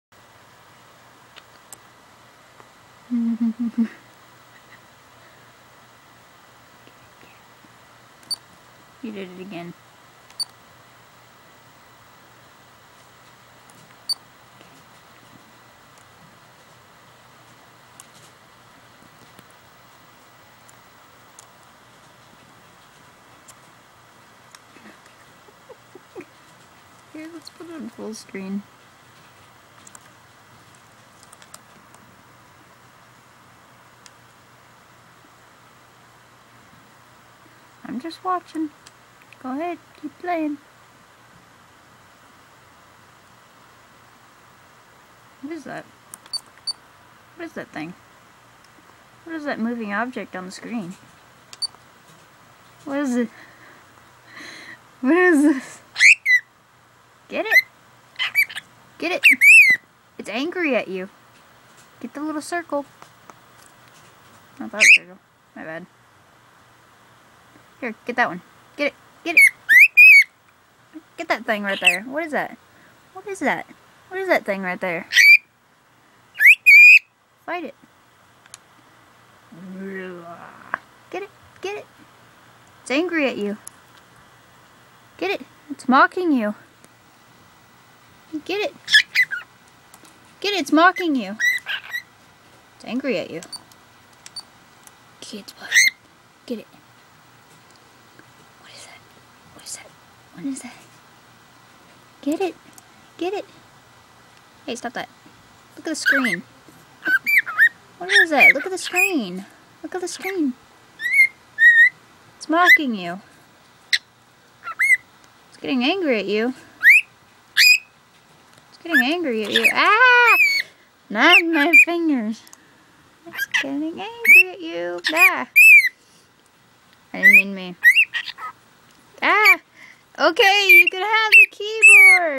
courser attack
stella attacking my mouse arrow on my computer